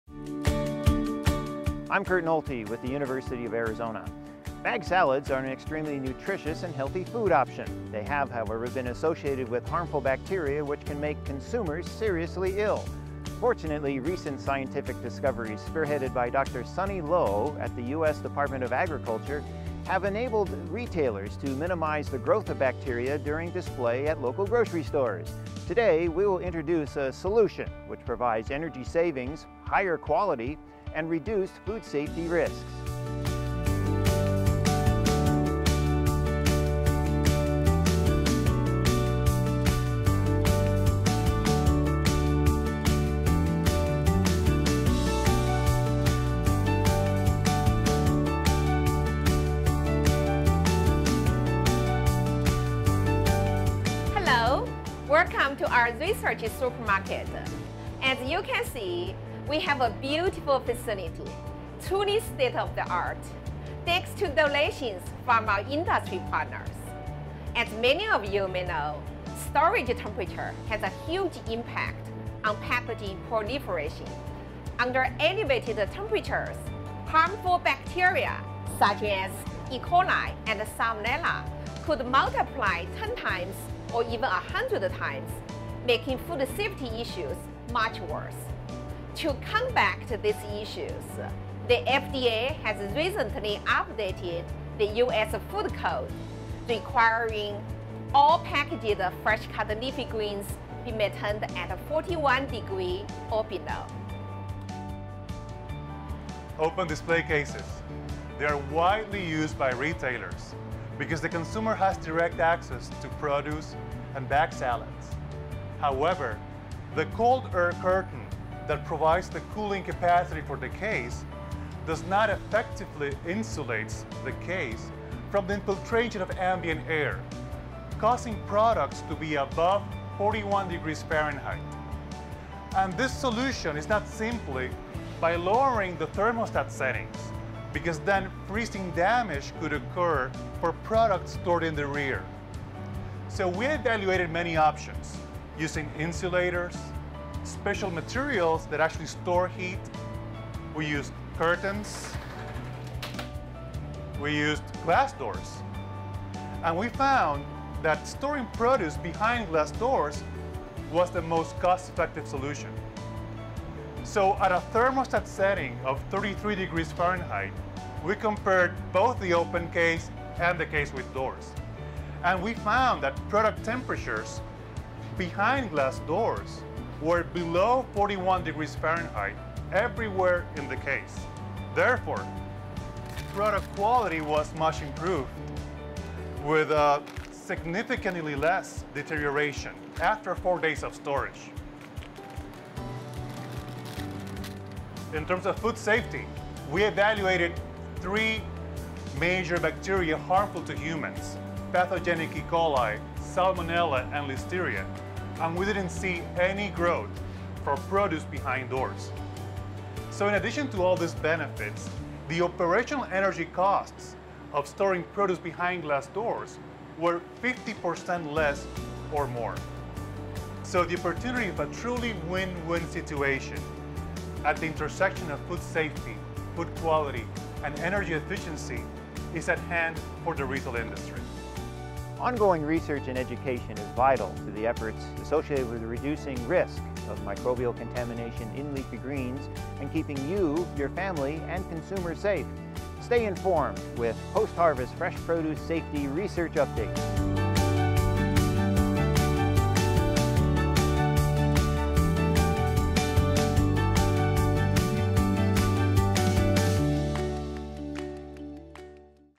Optimizing Fresh Produce Storage Condition
Dr. Luo and Dr. Atilio de Frias are sharing their innovative research on produce storage condition and their own recommendations on how produce should be stored in your grocery stores. The work is supported by a grant funded by the USDA, National Institute of Food and Agriculture. Innovative Technologies and Process Optimization for Food Safety Risk Reduction Associated with Fresh and Fresh-cut Leafy Green Vegetables, USDA-NIFA Specialty Crop Research Initiative Grant Award No. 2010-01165.